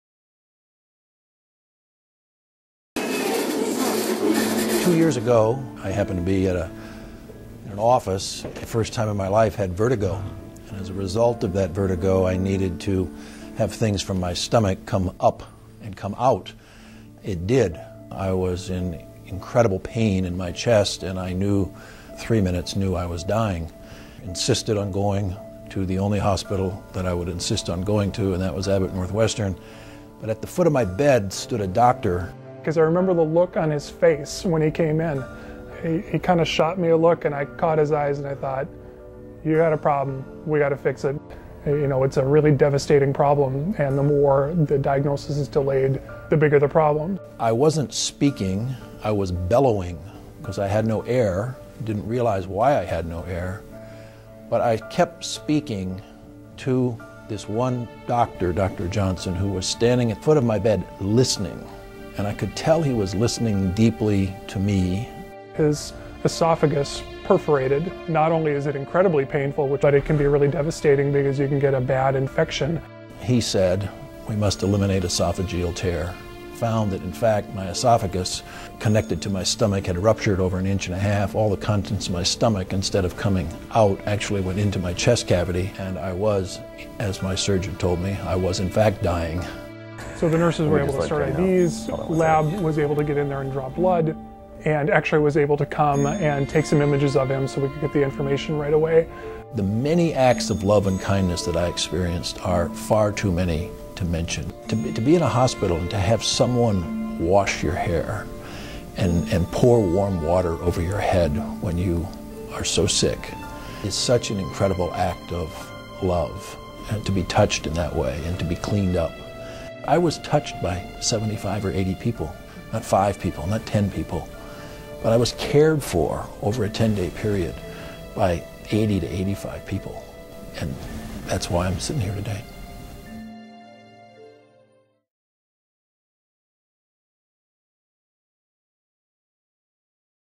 Countless acts of kindness help save a life
Bill Manning arrived at Abbott Northwestern Hospital with a life threatening esophageal tear. He credits his survival to the care he received in the Emergency Department, and the acts of love and kindness he experienced over a 10-day hospital stay.